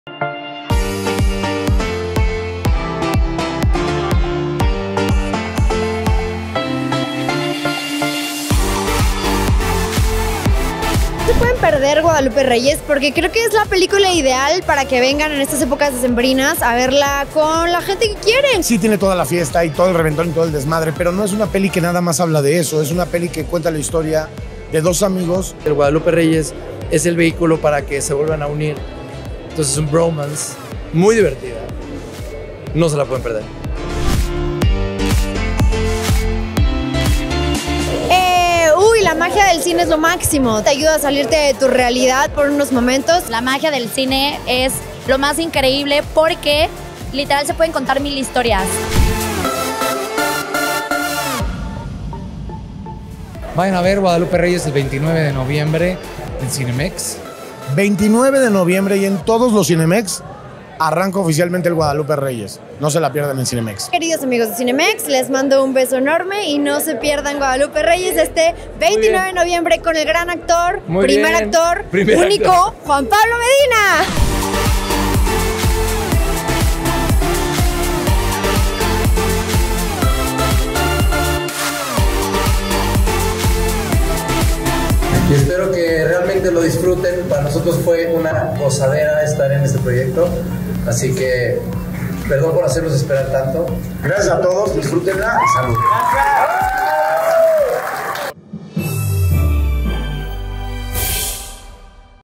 Guadalupe Reyes: Alfombra Roja
Así es como se vivió La Magia de la Alfombra Roja y Premiere de GuadalupeReyes.

Vive La Magia del Cine con esta divertida película y compra tus boletos